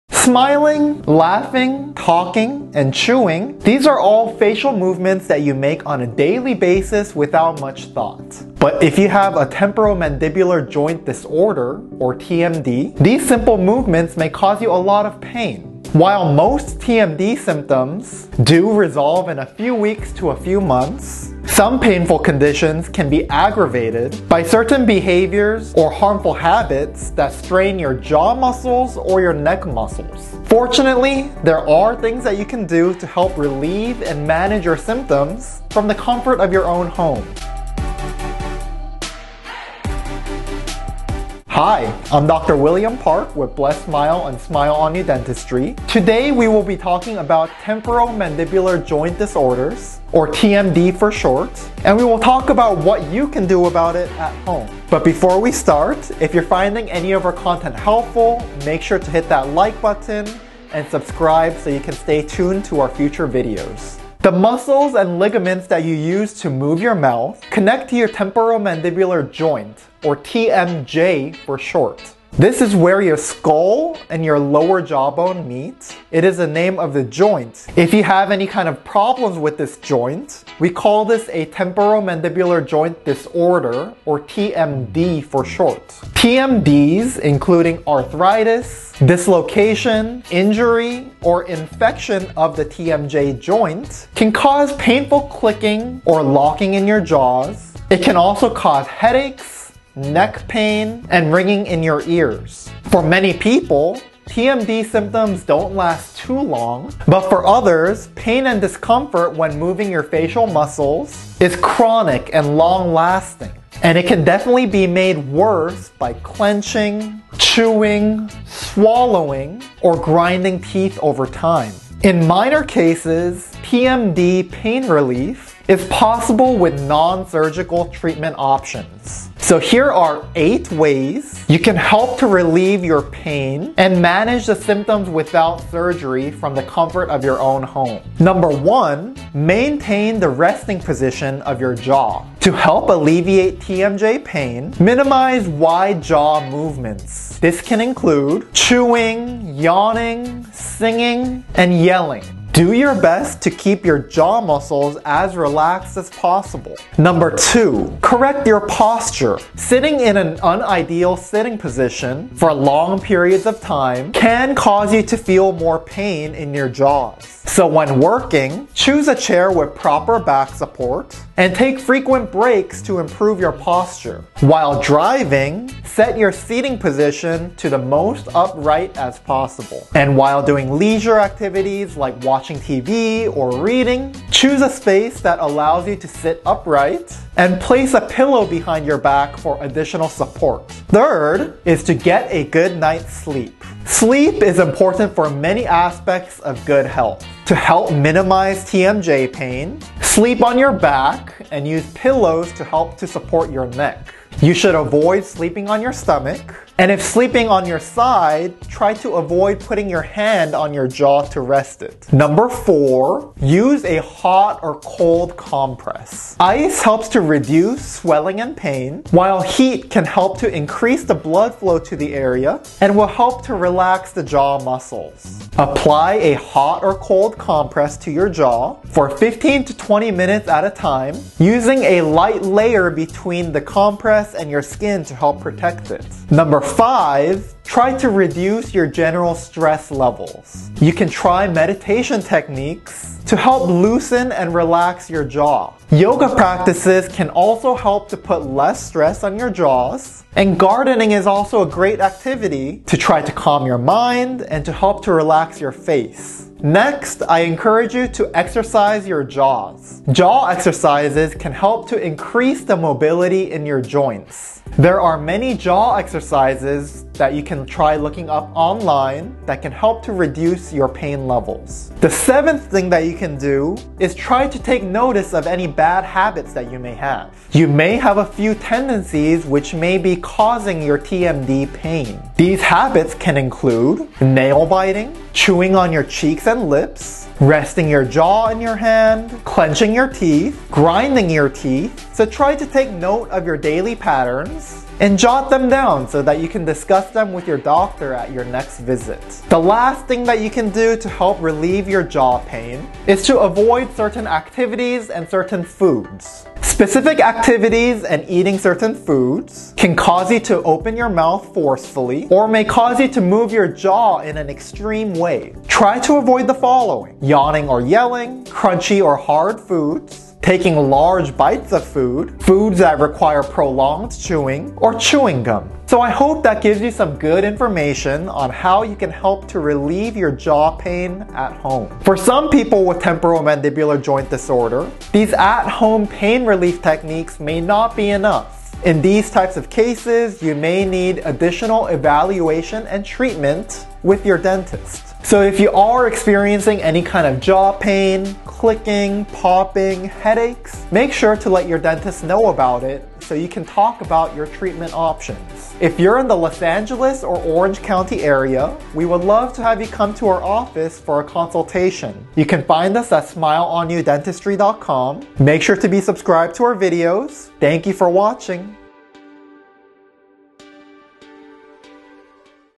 Treat TMJ Pain AT HOME | 8 WAYS TO GET RID OF JAW PAIN
You may not think twice about smiling, laughing, eating, or yawning, but when you start to have pain when you move your jaw, it can start to be very uncomfortable and concerning.

The muscles to move your mouth connect to your temporomandibular joints (TMJ), where your skull and lower jawbone meet. Temporomandibular disorders (TMDs) can cause painful locking/clicking in your jaw, headaches, neck pain and ringing in your ears.

In this video, Dr. William Park will discuss 8 things you can try doing at home to help relief your TMJ/jaw pain.

If you have any questions or comments on things you have tried at home to relieve your jaw pain, please let us know down in the comments!